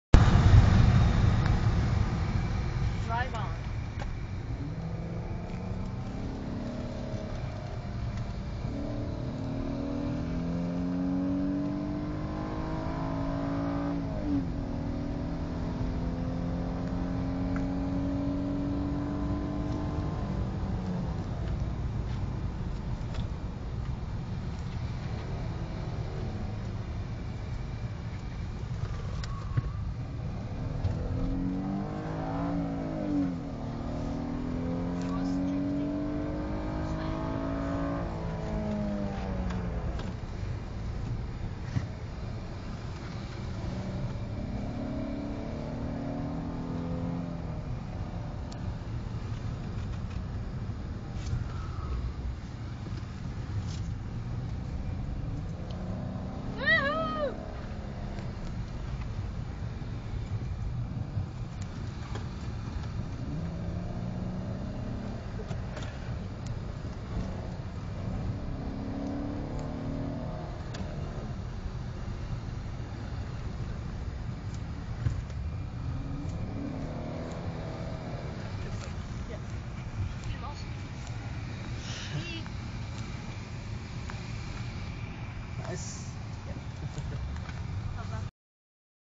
driving in stcc
my best friend santiago drove us in the swedish championships in car racing